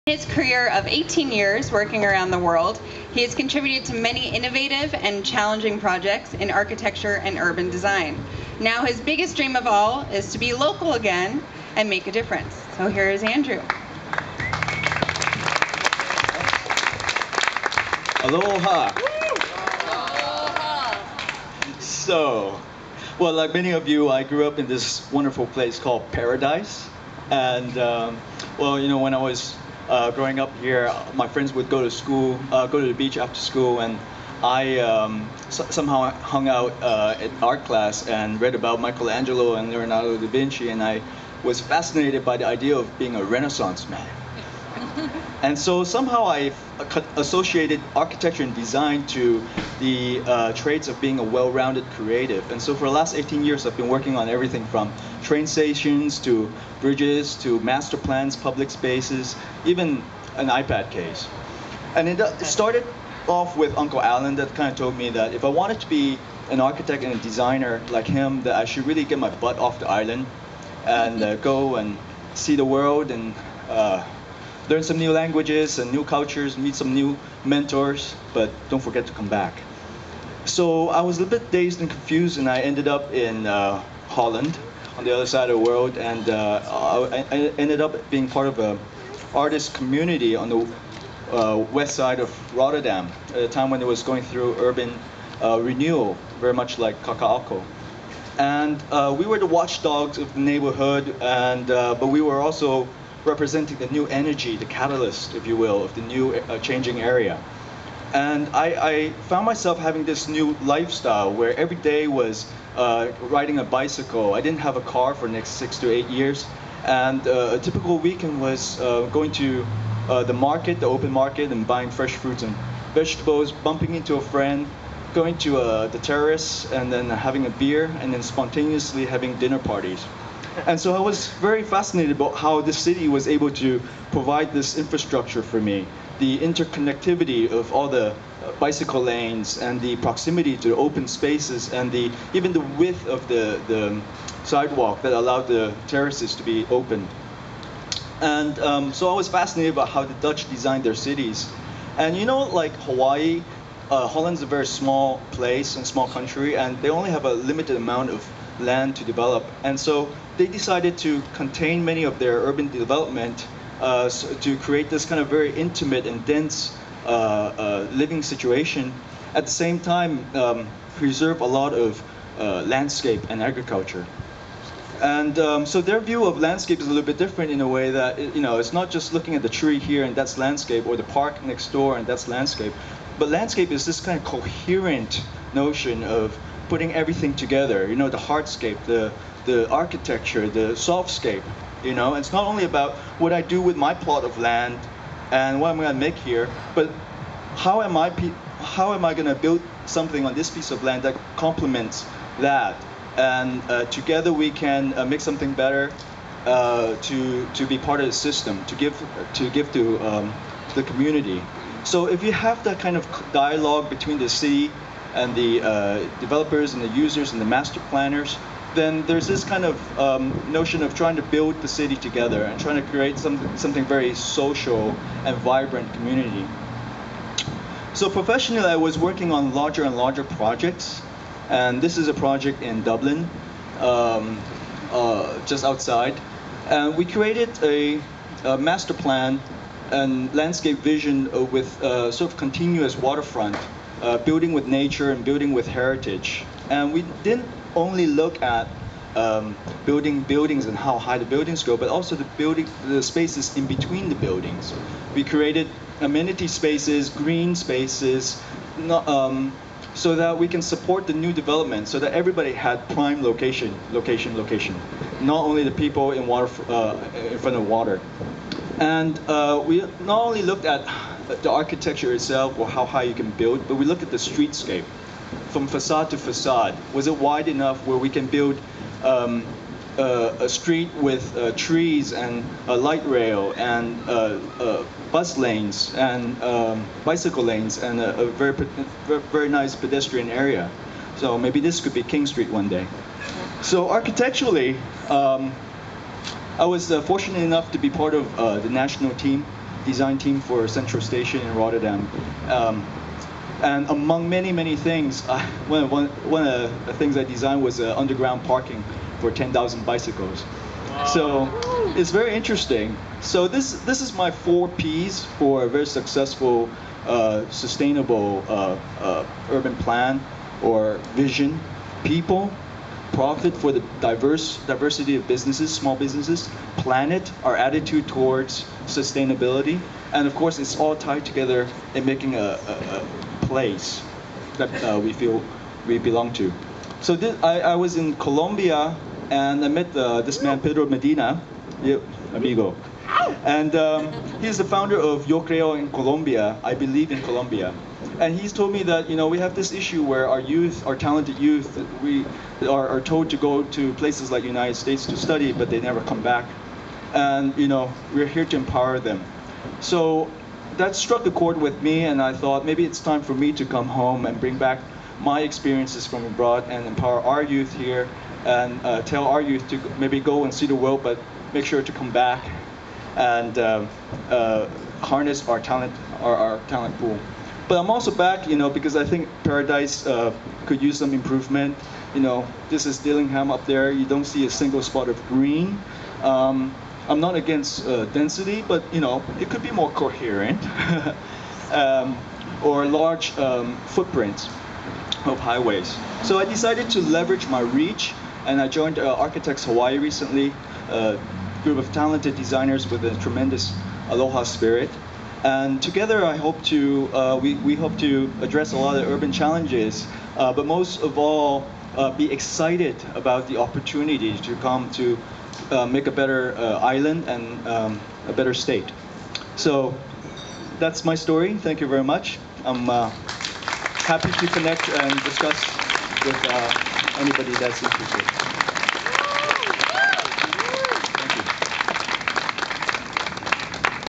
20130816-Andrew Tang speaks at Pecha Kucha Honolulu
Andrew speaks at Pecha Kucha in Honolulu to the theme of "Loco Local".  His talk, "Return of the Local", is a story of a Local boy leaving Hawaii, but comes back with new perspective about his home.  He is excited about passing on knowledge and contributing to the future of Hawaii.